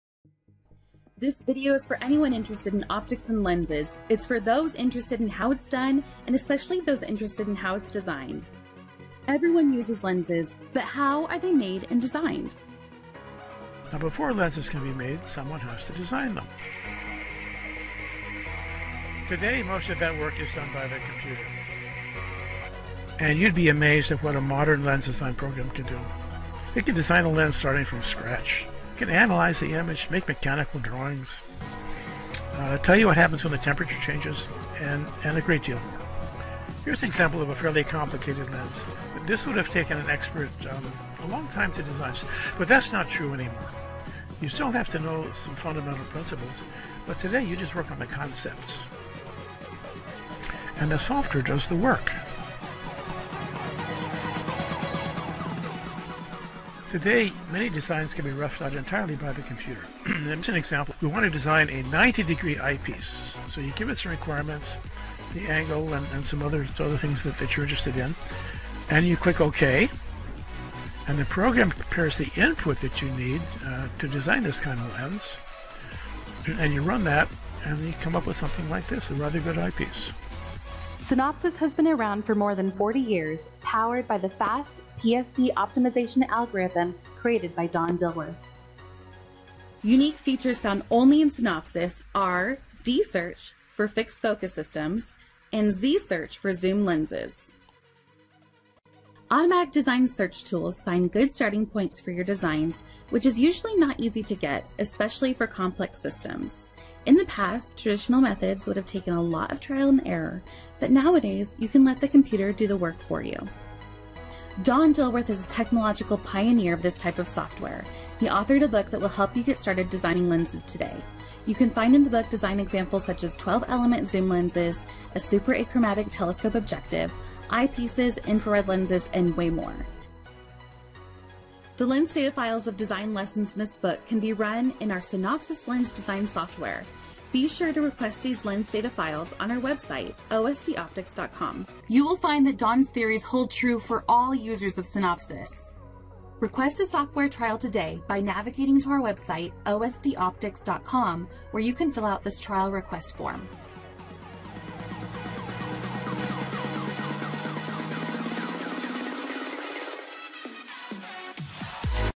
Lens Design Books and Software Created by Don Dilworth
Don Dilworth, the Creator of SYNOPSYS™ Lens Design Software, has authored multiple lens design books, including the Second Edition of ‘Lens Design, Automatic and quasi-autonomous computational methods and techniques', which was released Dec 2020.
His book
• Discusses Automatic Design Tools that can quickly produce excellent designs starting points and improve your designs
• Includes design examples such as 12 element zoom lens, a superachromatic telescope objective, eyepieces, infrared lenses, spy cameras, plastic aspheres, laser beam shapers, X-ray optics.

Request lens data file examples to use in SYNOPSYS™ alongside Don’s book